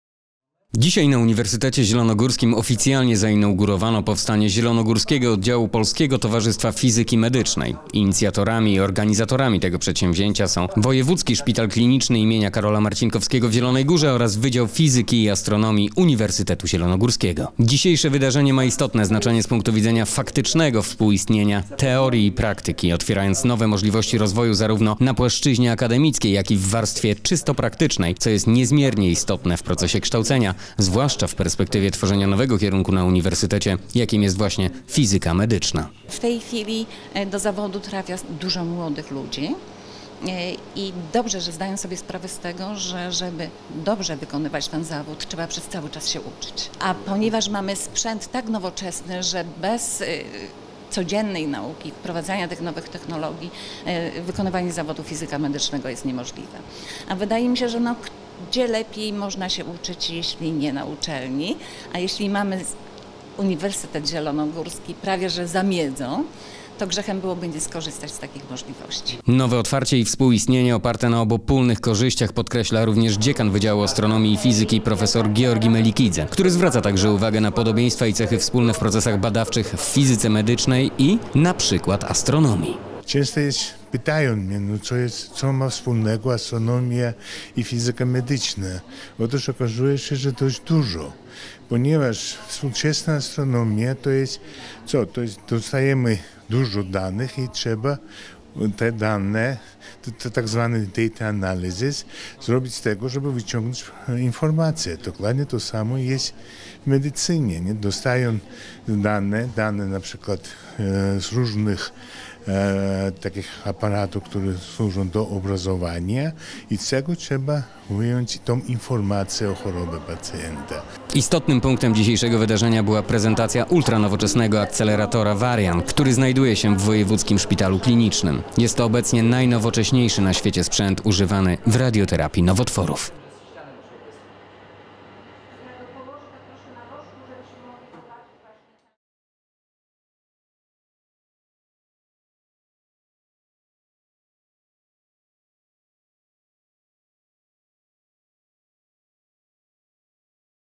Zielonogóski oddział Polskiego Towarzystwa Fizyki Medycznej
Dzisiaj na Uniwersytecie Zielonogórskim oficjalnie zainaugurowano powstanie zielonogórskiego oddziału Polskiego Towarzystwa Fizyki Medycznej. Inicjatorami i organizatorami tego przedsięwzięcia są Wojewódzki Szpital Kliniczny im. K. Marcinkowskiego w Zielonej Górze oraz Wydział Fizyki i Astronomii Uniwersytetu Zielonogórskiego.
Dzisiejsze wydarzenie ma istotne znaczenie z punktu widzenia faktycznego współistnienia teorii i praktyki, otwierając nowe możliwości rozwoju zarówno na płaszczyźnie akademickiej jak i warstwie czysto praktycznej, co jest niezmiernie istotne w procesie kształcenia, zwłaszcza w perspektywie tworzenia nowego kierunku na Uniwersytecie jakim jest właśnie Fizyka medyczna.
Istotnym punktem dzisiejszego wydarzenia była prezentacja ultranowoczesnego akceleratora Varian, który znajduje się w Szpitalu Klinicznym. Jest to obecnie najnowocześniejszy na świecie sprzęt używany w radioterapii nowotworów.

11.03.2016

Produkcja:
Telewizja Uniwersytetu Zielonogórskiego